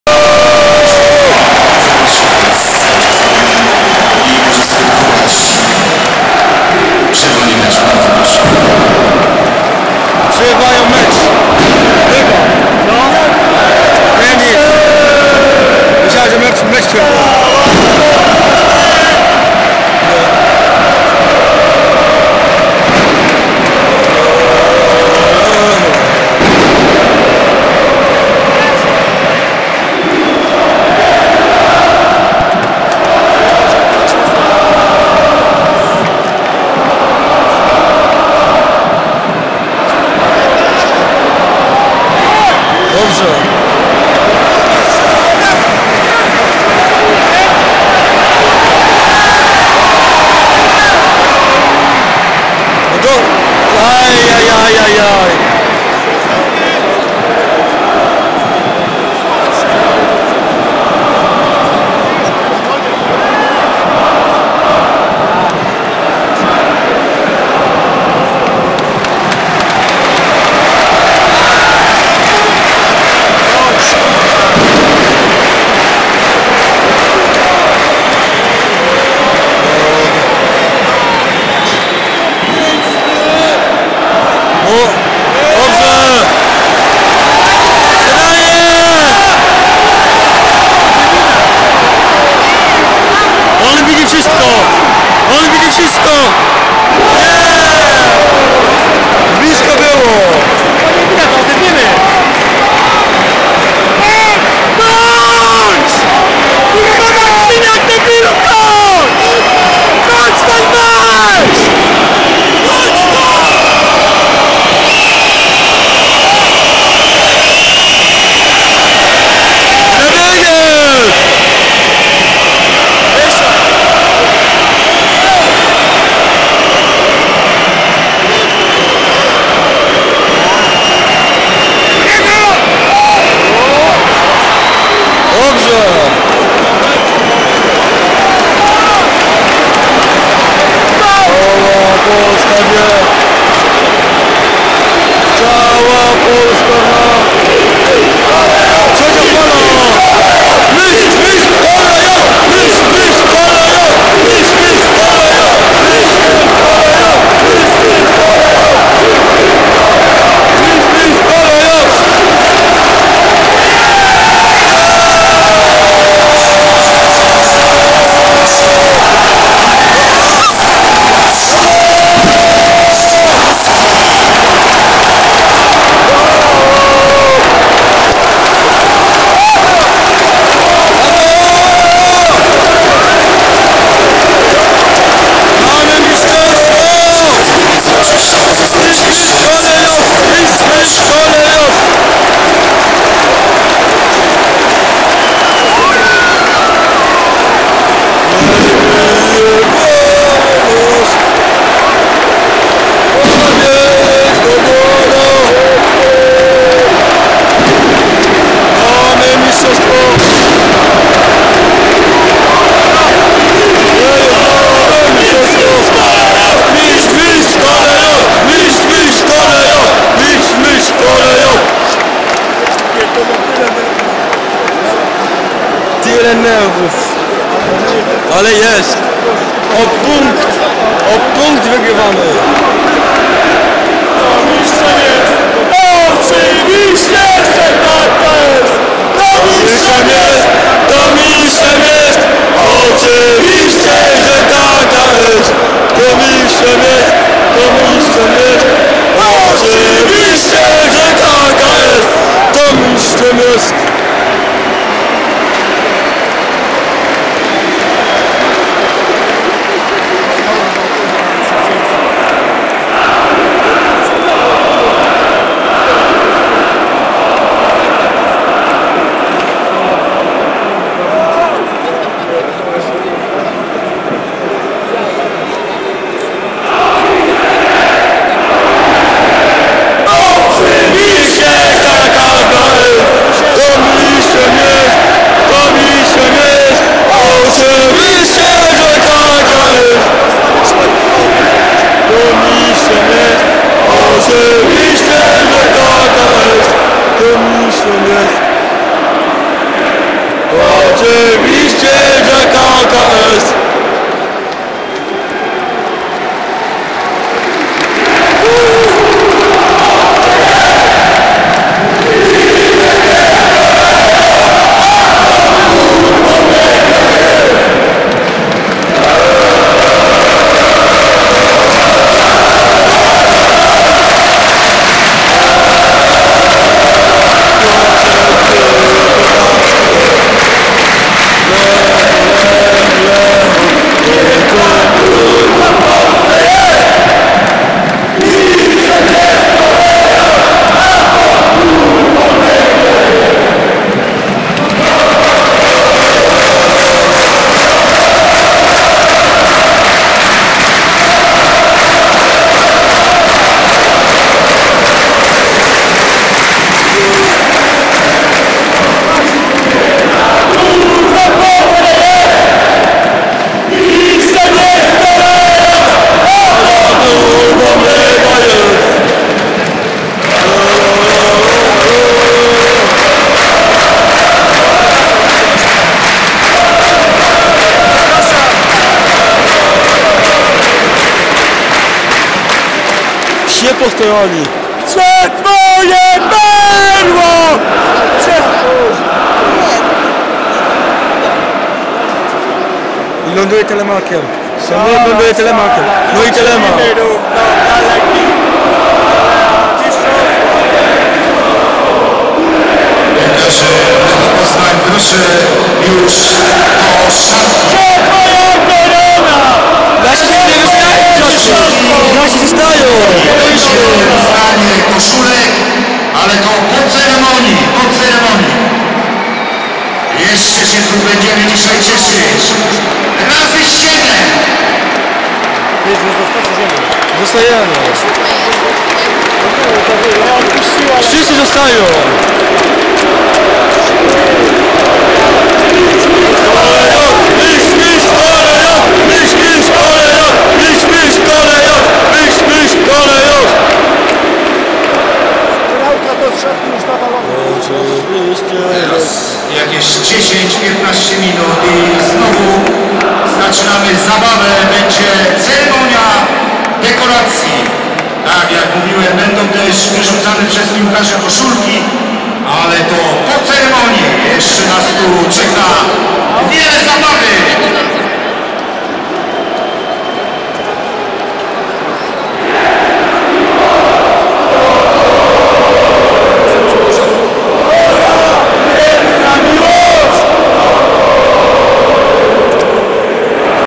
2 fala rac, ze strony kibiców Lecha, w walce o mistrzostwo Polski
Była to druga fala rac, odpalonych w czasie meczu Lech Poznań 0-0 Wisła Kraków. Pierwsze race odpalili tuż po rozpoczęciu meczu. (odsyłam do filmu, z pierwszej połowy meczu :) )